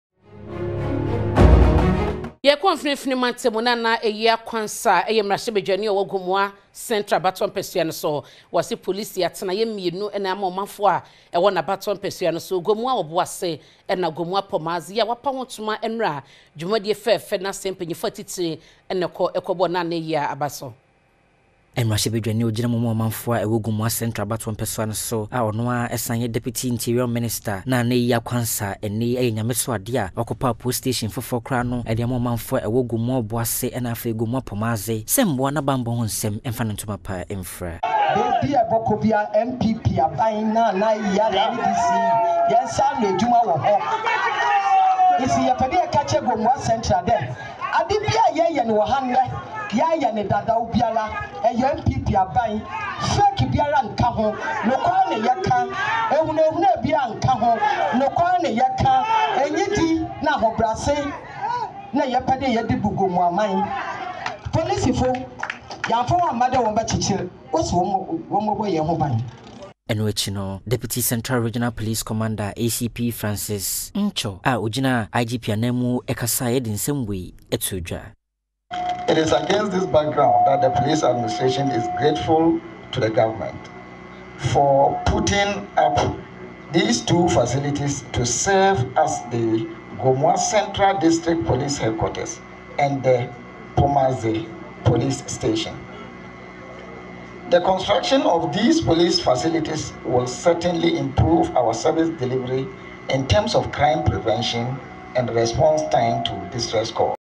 Enhancing Security: Gomoa Central MP commissions two police station at Gomoa Obuasi, Pomadze.
Enhancing Security: Gomoa Central MP commissions two police station at Gomoa Obuasi, Pomadze -Premtobre Kasee (29-10-24)